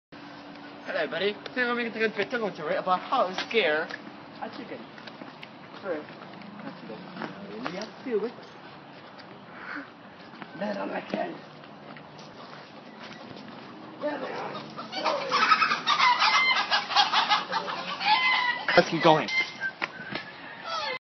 How to scare a chicken!
This is Sam from Silvr173 teaching you how to scare a chicken.